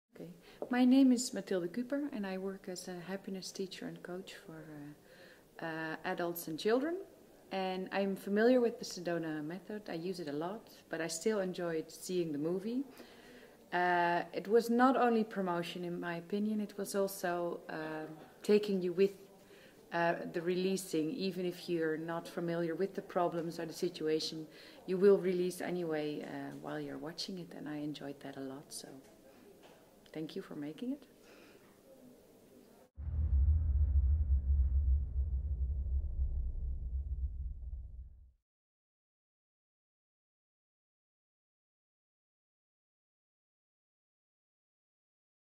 Mathilde Kuper
My name is Mathilde Kuper, and I work as a Happiness Teacher and Coach for adults and children. I am familiar with The Sedona Method, I use it a lot, but I still enjoyed seeing the movie. It was not only promotion in my opinion; it was also taking you with the releasing even if you're not familiar with the problems or the situation, you will release anyway while you're watching it, and I enjoyed it a lot. So, thank you for making it.